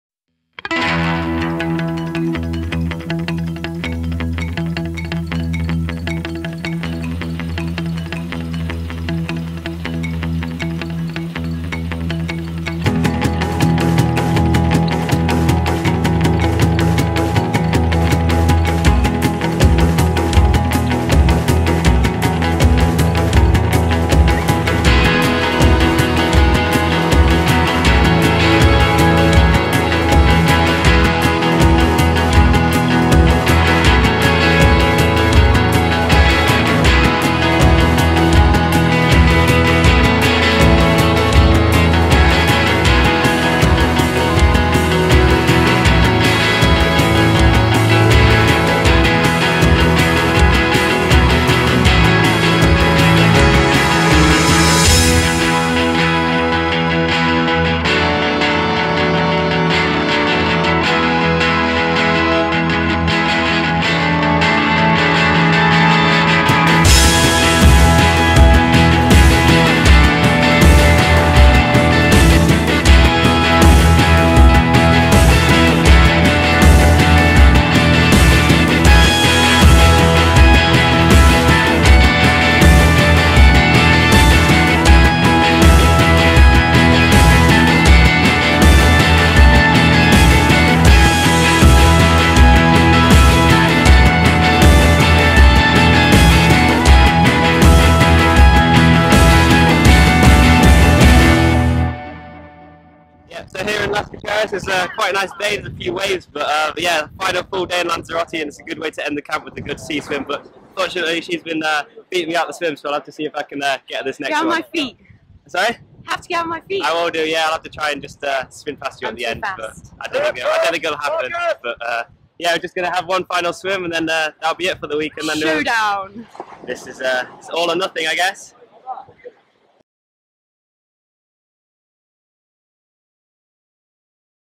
Triathlon England Academy Camp Lanzarote 2015
The East Midlands Regional Academy and North West Regional Academy are both part of the Triathlon England Talent Development Programme. This is a short video clip of their camp at Sands Beach Resort Lanzarote, Canary Islands.